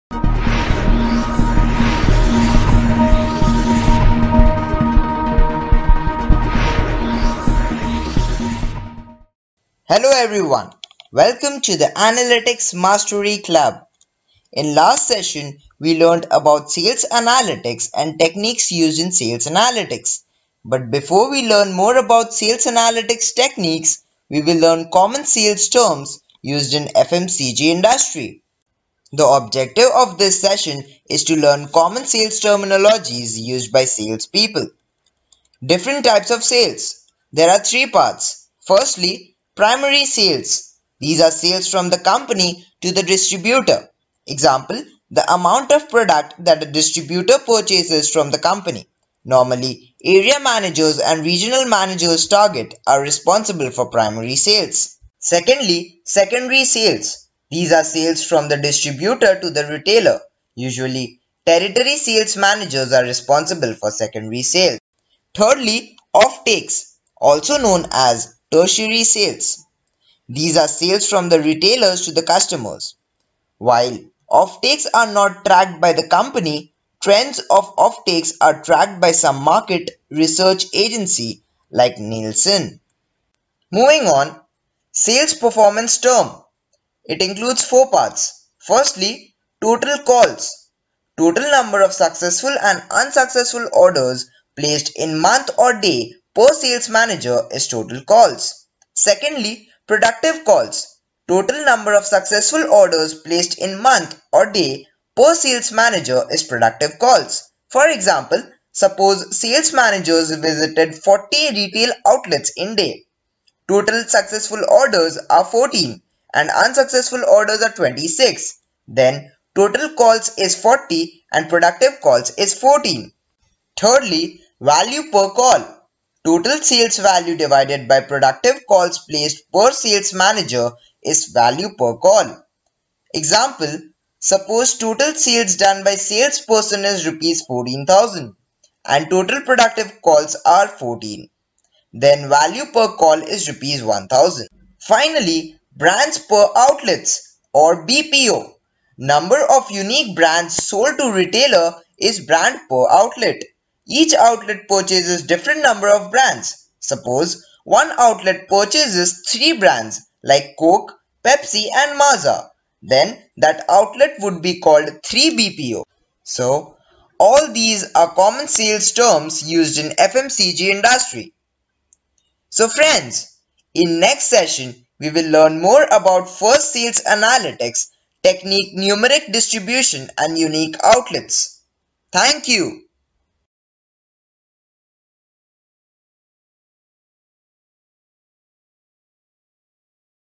Analytics Mastery Session 11 : Sales Analytics Terminologies
Top comments for last case study are from following participants :
1) Ayush Sinha
2) Harsha Chaturvedi and Sushmit Sharma
3) Aditya Gupta

Winner for top comment of day is : Ayush Sinha

Objective of this session is to learn common sales analytics terminologies used by sales people